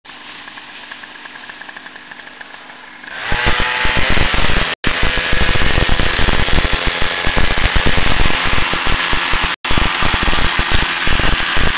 Tailwind Powered Paraglider Launch
I was told it was impossible to "reverse" launch a powered paraglider in no wind. Having done it before, I set out to prove otherwise. Using the cross-armed technique covered in the PPG Bible, it was possible in up to a 1mph tailwind component with an easy to inflate wing. The wind was a quartering tailwind from my right (launching 180, wind from 300 at 3)